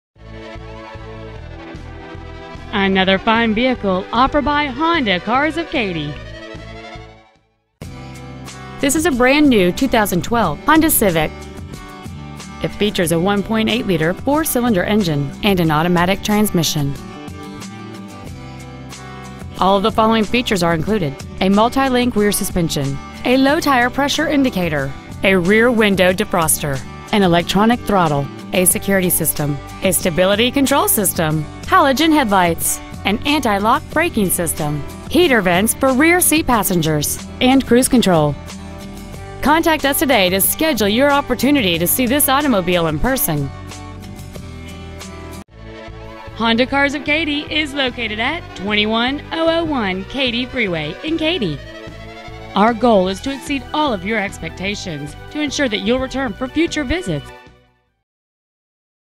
2012 Honda Civic Katy TX
Year : 2012
 Make : Honda
 Model : Civic
 Engine : 1.8L SOHC MPFI 16-valve i-VTEC I4 engine
 Trans . : Automatic
 Exterior : Gray

 Interior : Gray

 2012 Honda Civic Katy TX